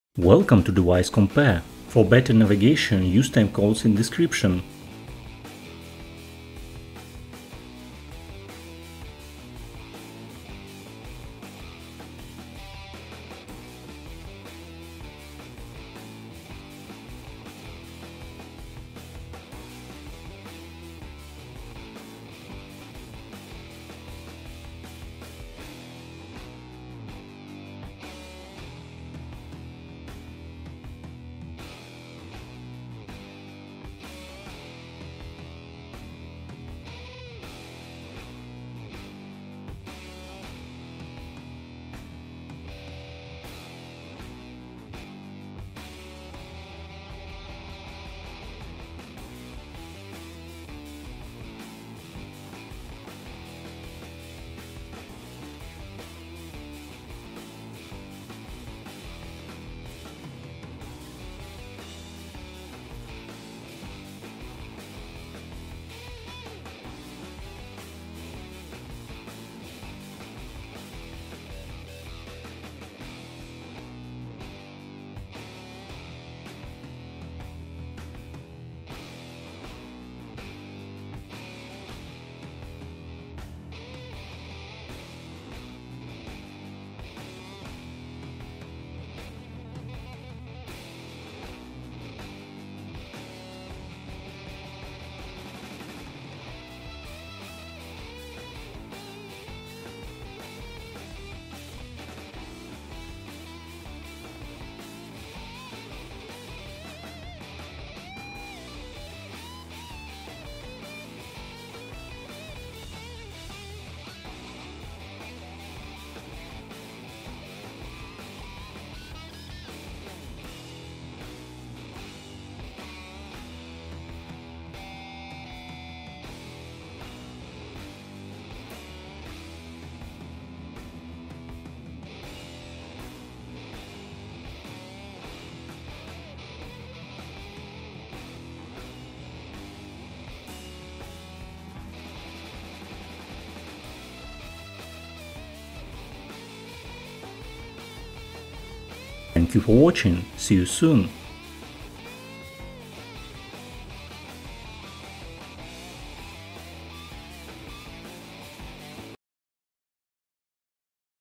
Xiaomi Mi 9T vs Xiaomi Redmi Note 7: Comparison
We compare Xiaomi Mi 9T and Xiaomi Redmi Note 7. Find out which is better and their overall performance in the smartphone ranking 2020

00:00 Intro (Pros and cons)
00:30 Performance
00:46 Design
01:02 Display
01:18 Primary camera
01:33 Front-camera
01:46 Feautres
01:59 Battery
02:16 Storage